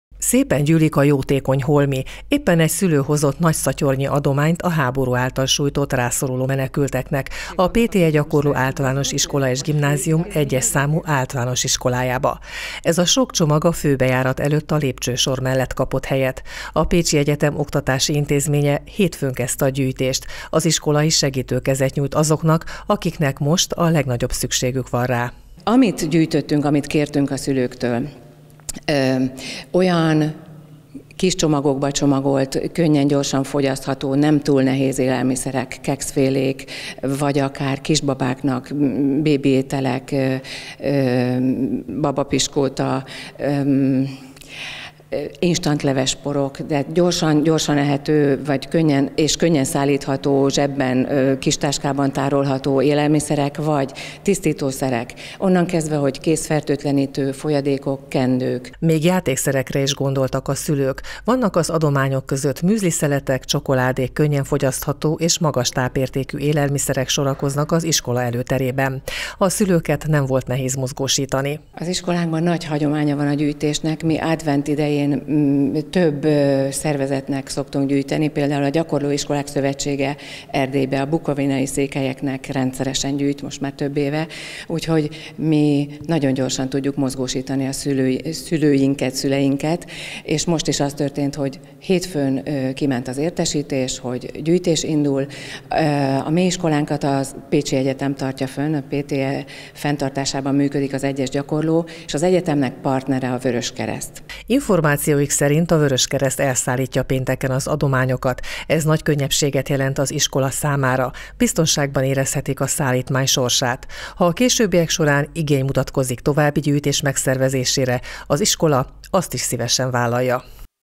Adományt gyűjtenek a pécsi iskolák
A héten több pécsi iskola is adománygyűjtésbe kezdett a háború elől menekülők megsegítésére. A PTE Gyakorló Általános Iskola és Gimnázium 1. számú Általános Iskolája tartós élelmiszert, azonnal fogyasztható ételeket, takarókat, higiéniai termékeket gyűjtött a szülők segítségével. Az adományokat csütörtök estig fogadják, melyeket a hétvégén a Vöröskereszt elszállít a magyar határ közelében lévő segítségpontokhoz az Ukrajnából menekülők részére.